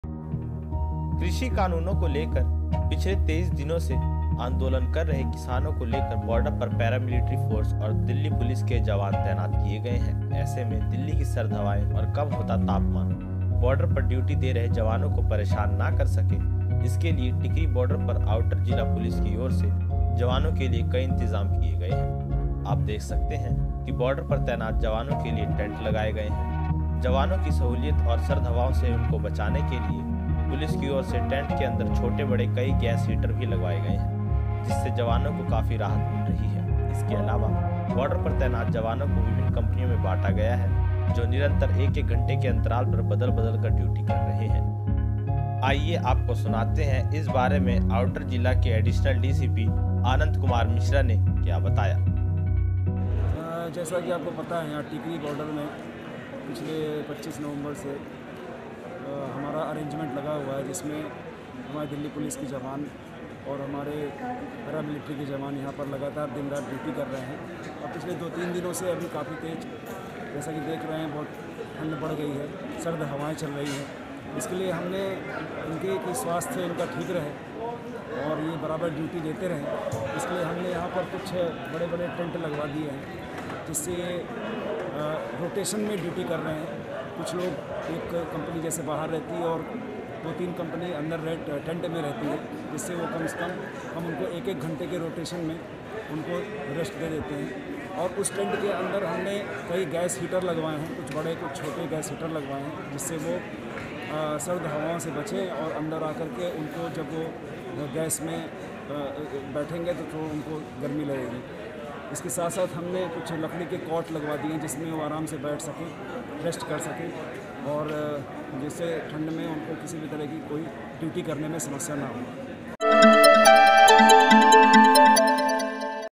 दिल्लीः टीकरी बॉर्डर पर किसानों का आंदोलन 23 वें दिन भी जारी
कृषि कानून को लेकर पिछले 23 दिनों से आंदोलन कर रहे किसानों को लेकर बॉर्डर पर पैरामिलिट्री फोर्स और पुलिस दिल्ली पुलिस के जवान तैनात किए हैं. ऐसे में दिल्ली की सर्द हवाएं और कम होता तापमान बॉर्डर पर ड्यूटी दे रहे जवानों को परेशान ना कर सके. इसके लिए टिकरी बॉर्डर पर आउटर जिला पुलिस की ओर से जवानों के लिए कई इंतजाम किए गए हैं.आप देख सकते हैं की बॉर्डर पर तैनात जवानों के लिए टैंट लगाए गए हैं. जवानों की सहूलियत और सर्द हवाओं से उनको बचाने के लिए पुलिस की ओर से टेंट के अंदर छोटे-बड़े कई गैस हीटर लगवाए गए हैं. जिससे जवानों को काफी राहत मिल रही है. इसके अलावा बॉर्डर पर तैनात जवानों को विभिन्न कंपनियों में बांटा गया है. जो निरंतर एक-एक घंटे के अंतराल पर बदल-बदल कर ड्यूटी कर रहे हैं.इतना ही नहीं ड्यूटी दे रहे जवानों के आराम करने के लिए वहां लकड़ी के बेड भी टैंट के अंदर लगवा दिए गए हैं. ताकि ड्यूटी देकर थके जवान आराम कर सकें. और फिर जब ड्यूटी आये तो बिना थकान ड्यूटी करें. बॉर्डर पर मौजूद जवानों को सर्दी से बचने के लिए किये गए इस इंतजाम से राहत मिल रही है. इस तरह की सुविधा उपलब्ध होने से जवान रोटेशन करके आसानी से ड्यूटी दे रहे हैं.